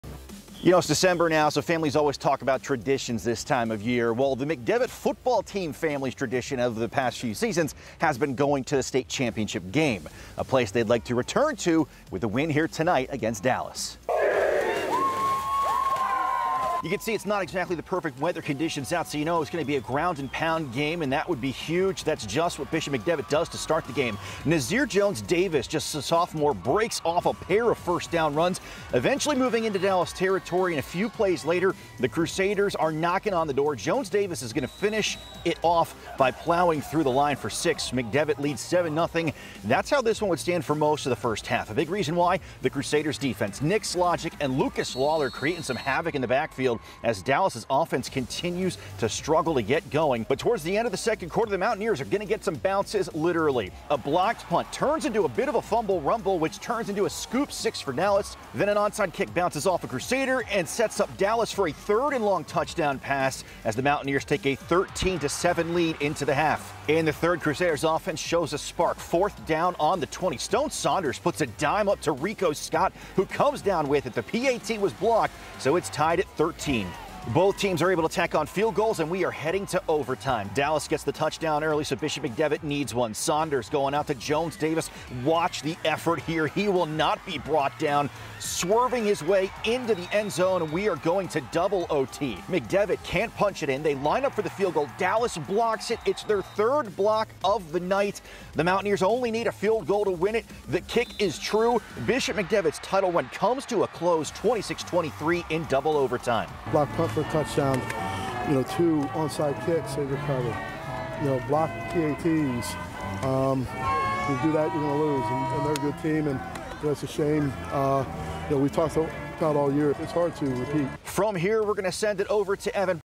Bishop McDevitt falls in double-overtime to Dallas, 26-23
Bishop McDevitt continued its chase for a second straight PIAA Class 4A title Friday night when the undefeated Crusaders faced District 2 champion Dallas in a battle of the unbeaten.